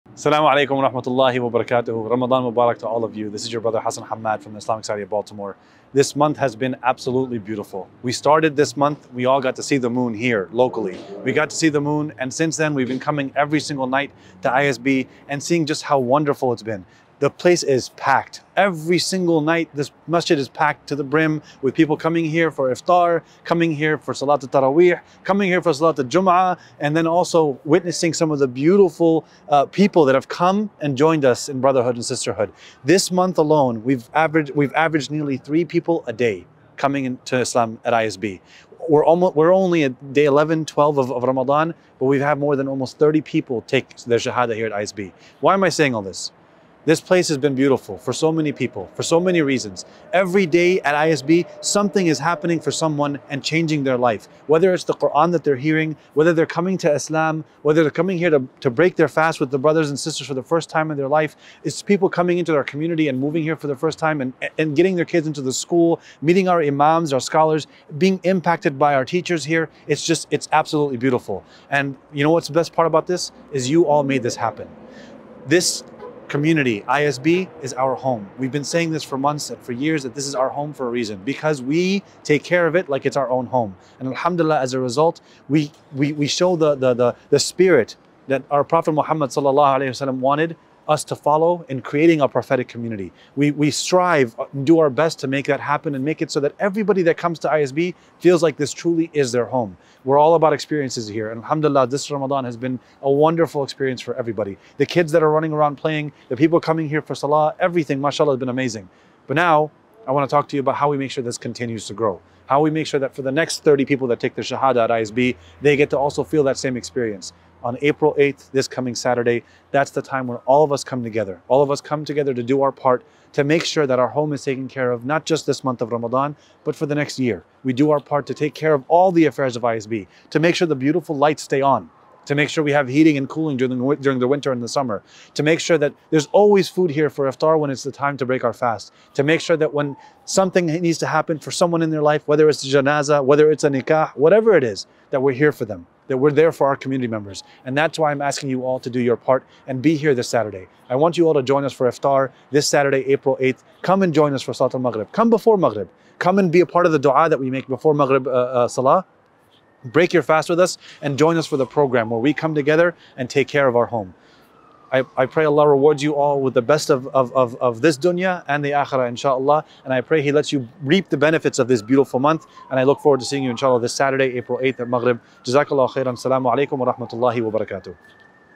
A Message From Our President | ISB My Home Benefit Iftar 2023
You’re invited to join the community THIS Saturday April 8th at 6:30pm at the largest Iftar in the DMV area- the My Home Benefit Iftar.

“You’ve been doing a very great job MashaAllah,” said ISB’s Sh. Ismet Akçin, “You are the one who took care of your own home all these years, past 50 years. Without you, no single activity can be run.”

“This is your home and the home of every single Muslim,” said ISB’s Sh. Yaseen Shaikh, “By the grace and mercy of Allah, so many people have to come to Islam, so many people are growing in their faith and growing in their Deen, and making ISB their home.

Come and find out on that night what we’ve accomplished in the past year, how you’ve been a part of all of that, and how you can continue to be a pillar of support.”

We’re excited to see you there. Meet you at your place - ISB, your home.